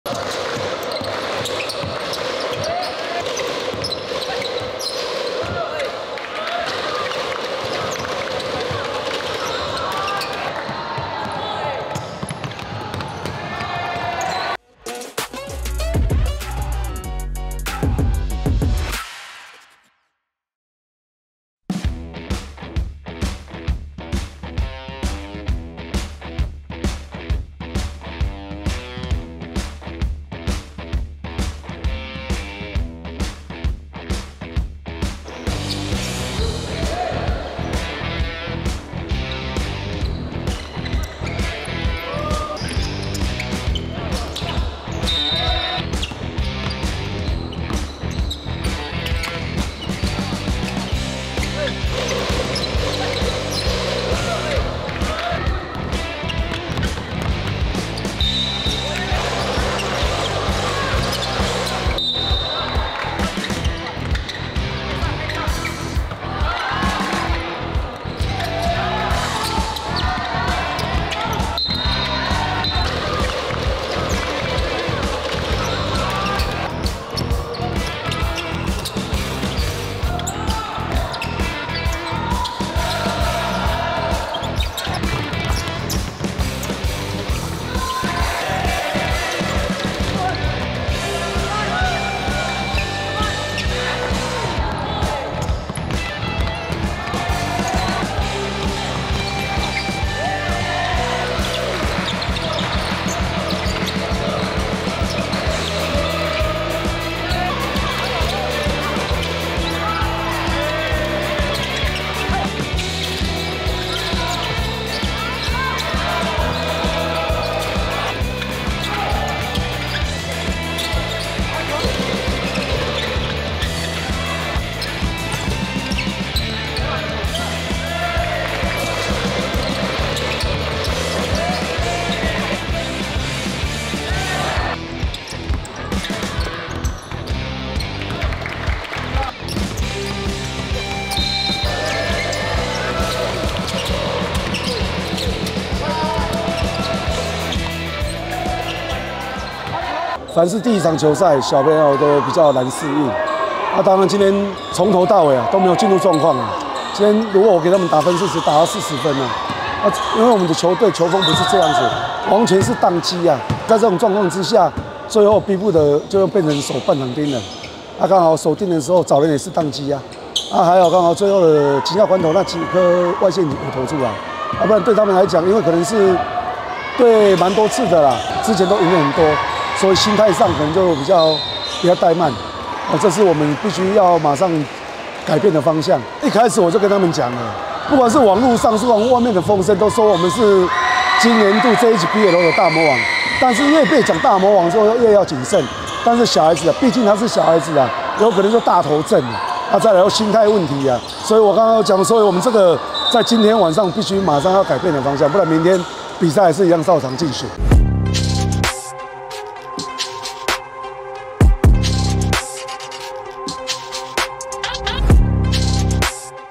【112JHBL男子資格賽_彰化溪湖】 外界評為大魔王 首場教練卻只評40分 認球員心態需調整 ｜Ft. 尤世銘｜JHBL國中籃球聯賽
彰化溪湖資格賽首場對上中市龍津
意外陷入拉鋸戰
半場結束更是由中市龍津領先
教練認為本場賽事僅有40分
球員因為外界評價以及過去和中市龍津的對戰經驗而鬆懈
產生心態問題
球隊需要儘速調整狀況
並且打出溪湖該有的球風